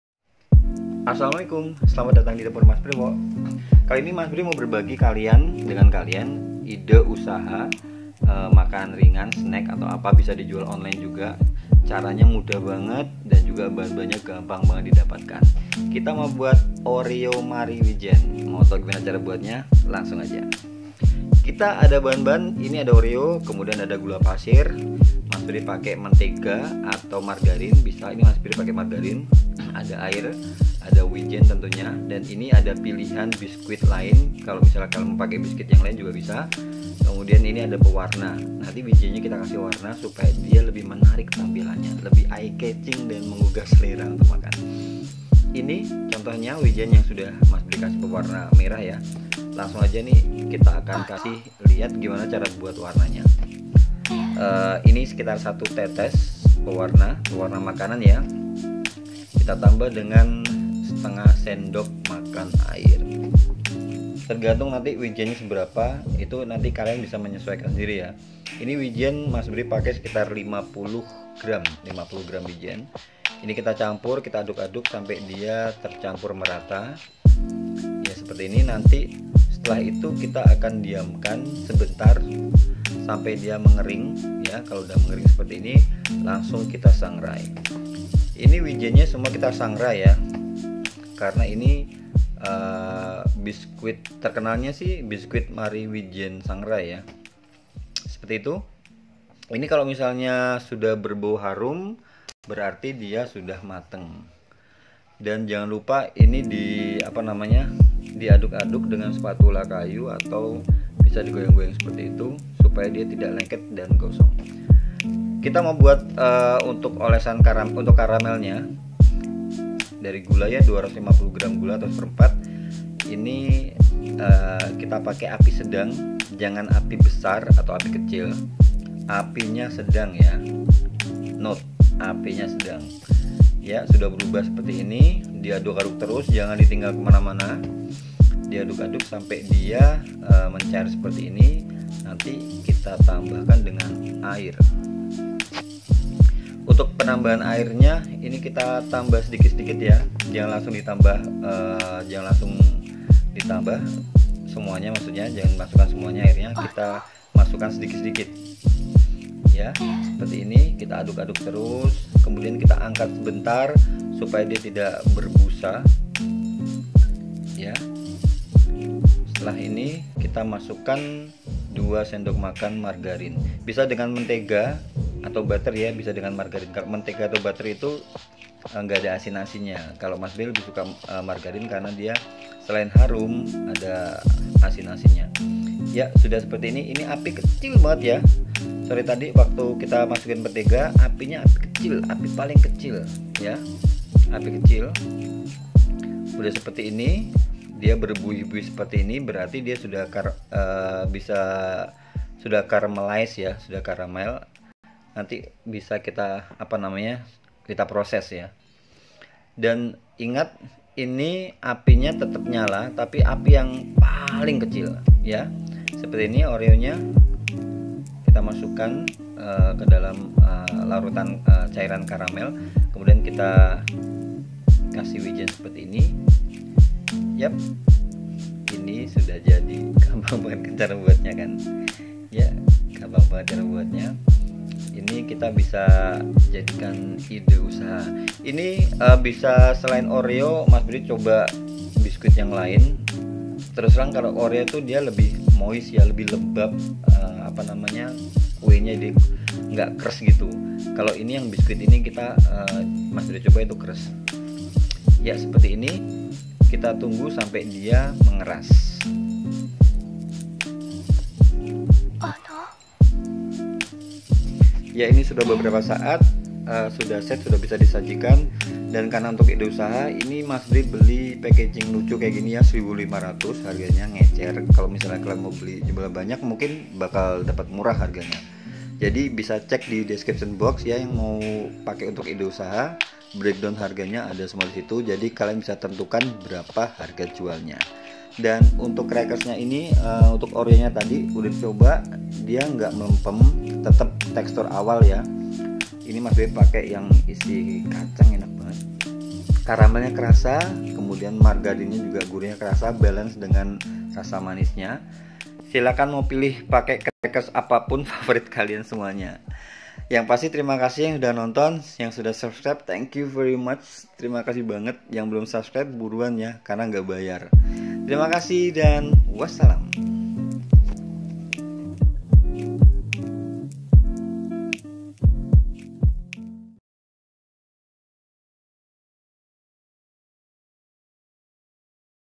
MARIE WIJEN DARI OREO | IDE USAHA KECIL
Yuk yg mau buka lapak jualan
Ini bisa jadi ide usaha
Terinspirasi dari Snack jadul Marie wijen

Kita mau buat Oreo Marie wijen

Bahan bahan ::
Biskuit Oreo 8000

Bahan karamel + wijen
Gula 259gr. 3500
Margarine 2sdm 1000
Gas . 3000
Air 50ml. . Gratis
Pewarna makanan pakai yg sdh ada

Wijen 250gr . 12500
Total pengeluaran . 20000

ukuran bahan di atas bisa utk sekitar 40keping biskuit (ukuran biskuit Oreo)

Jadi kalo pake packaging spt contoh video (6keping) bisa dibuat jadi 6 sd 7paket mika

20.000 : 6mika (isi 6) = 3.333 dibulatkan 3500

Oreo 8000 (isi 14keping) = 571 dibulatkan mjd 600/keping

kemasan mika kecil isi 6keping, modal dasarnya = ( 600 x 6) = 3600 + 3500+1500(mika) = 8600

Jadi modal per mika kemasan isi 6 = 7100
Jgn lupa ditambah tenaga kita buat 1400
Jadi total modal bahan dan tenaga 10.000

Biasanya keuntungan diambil 30% sd 50%
Jadi harga jualnya 13rb sd 15rb/kemasan isi 6

Apabila kalian menggunakan biskuit lainnya yg lbh murah mungkin bisa diturunkan lagi harganya

Ok, segitu dulu video ide jualan kali ini
Semoga anda terinspirasi dan sukses

Aamiin

Salam masBrewok